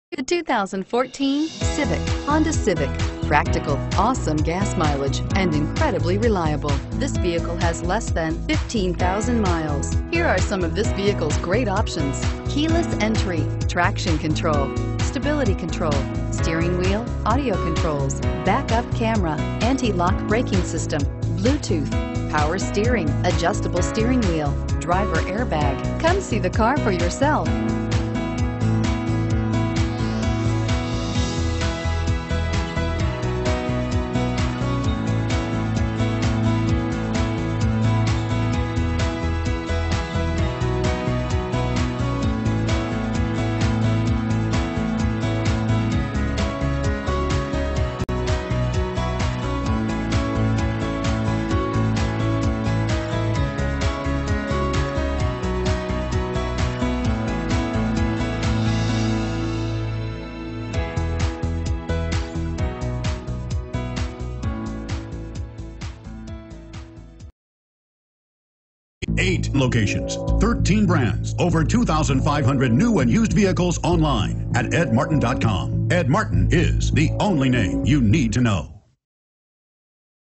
2014 Honda Civic Sedan 2X1509
2014 HONDA CIVIC SEDAN Indianapolis, IN
Stock# 2X1509

HONDA CERTIFIED!, BALANCE OF FACTORY WARRANTY!, ONE OWNER LOCAL TRADE!, SERVICED & COMPLETE DETAIL!!, And GREAT SERVICE HISTORY!. This fantastic 2014 Honda Civic is the car that you have been trying to find. This wonderful Honda is one of the most sought after used vehicles on the market because it NEVER lets owners down. Honda Certified Pre-Owned means you not only get the reassurance of a 12mo/12,000 mile limited warranty, but also up to a 7yr/100k mile powertrain warranty, a 182-point inspection/reconditioning.